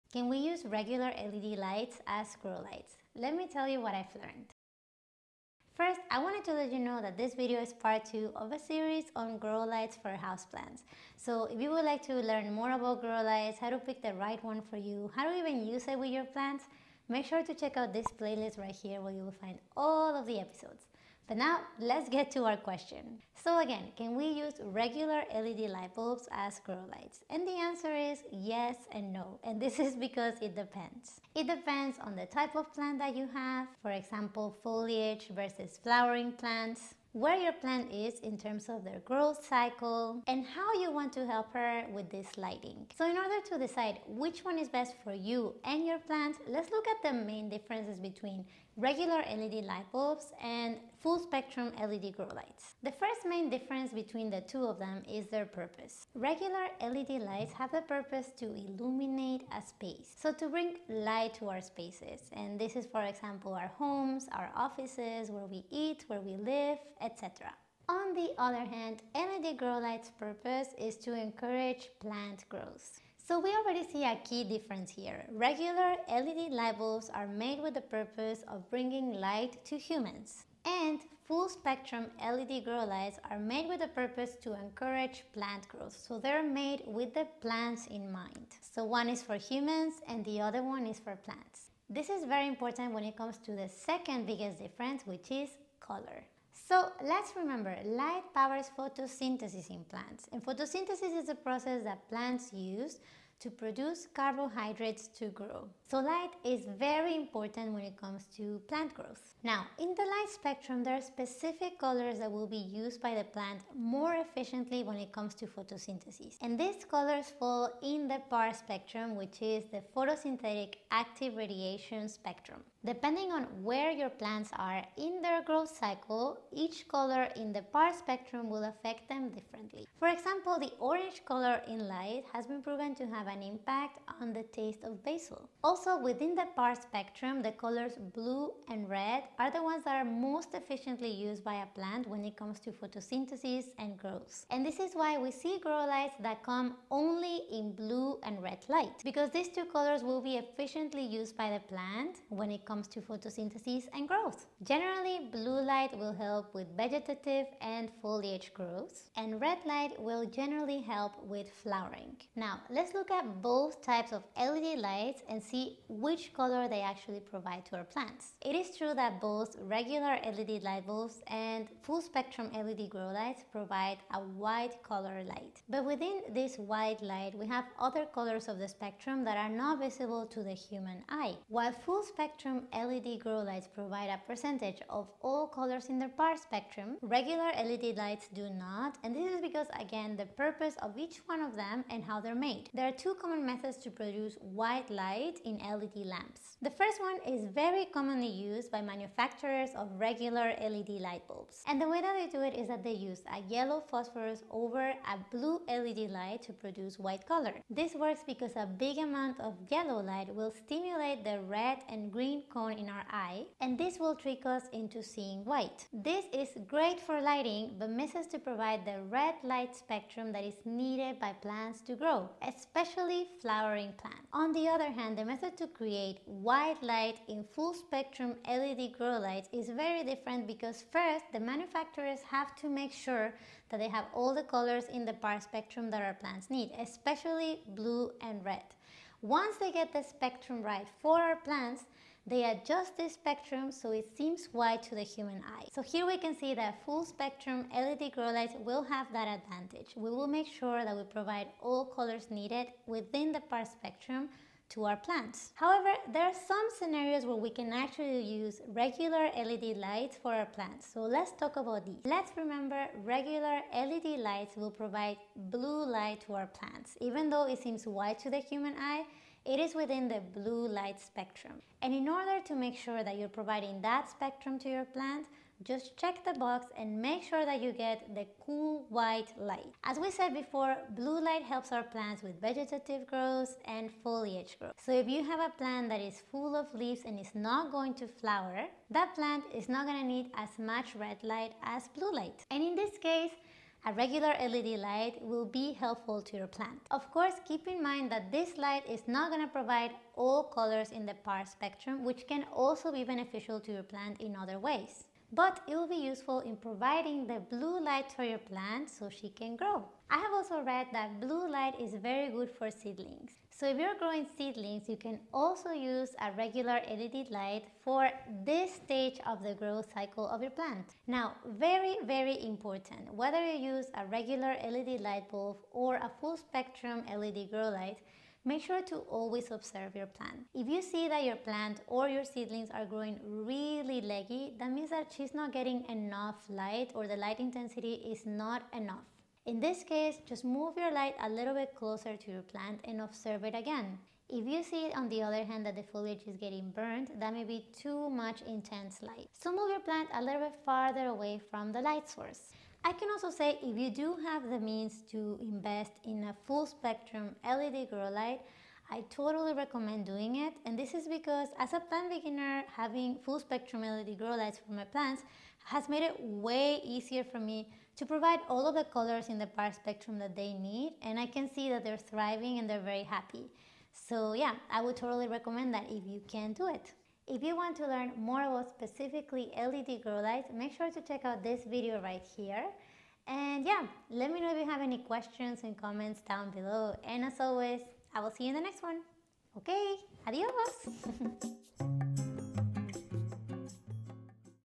Regular LED lights for plants : GROW LIGHTS FOR HOUSEPLANTS (PART 2)
In this video we go over grow lights vs led lights (regular led light bulbs) and whether we can grow plants with LED lights. You can find my favourite full spectrum LED grow lights here:

Affordable LED grow light for seedlings

Full Spectrum LED grow light (my favourite now -2022) PlantSpectrum32 ! and using the code PLANTSANDLUCIA

TIMESTAMPS:
0:00 - Introduction
0:22 - Can we use regular LED lights as grow lights?
0:42 - LED Grow lights vs LED lights (purpose)
1:31 - LED Grow lights vs LED lights (colours)
4:15 - When we can use regular LED lights as grow lights
5:20 - How to know if plants are getting the right light

ABOUT THIS VIDEO: Regular LED lights for plants : GROW LIGHTS FOR HOUSEPLANTS (PART 2)
Can I use LED lights as grow lights? That is a question I get super frequently. So I thought this video on regular LED lights as grow lights would be perfect for this time of year :-).In the video we will go over grow lights vs led lights and the ways in which is is possible to use regular LED lights as grow lights. Using LED lights to grow plants is possible in some cases, and of course in these cases it can be very beneficial as regualr led lights tend to be more afforadable. After watching the video you will know when to grow plants with LED lights (the regular led light bulbs) and when it is better to stick to the full spectrum grow lights. I hope this video help you in deciding when to use LED lights for plant growth especially when you are thinking about LED lights for indoor plants :-)